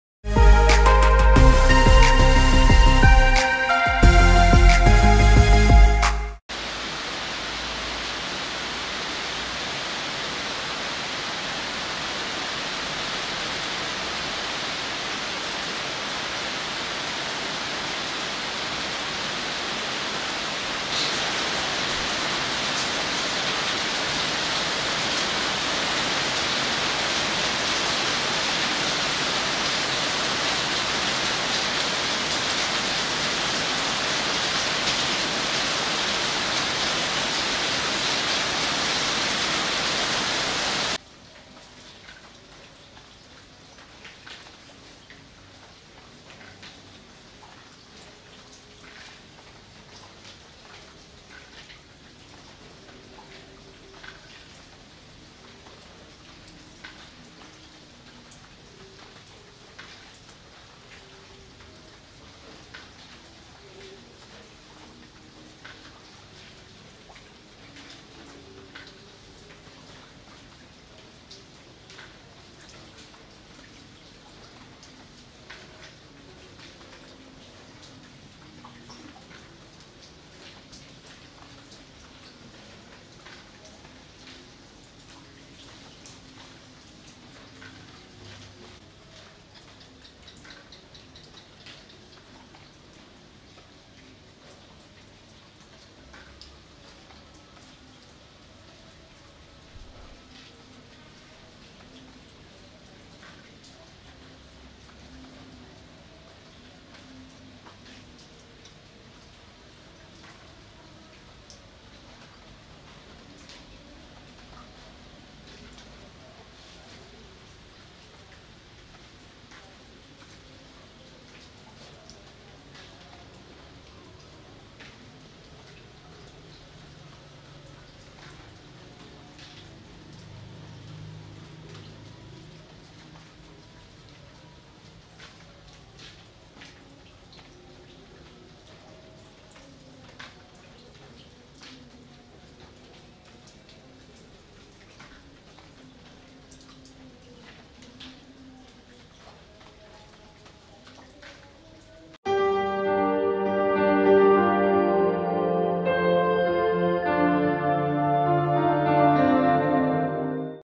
Sisa Air Hujan Di Daun
Tetesa air sisa hujan pada bunga tropis ..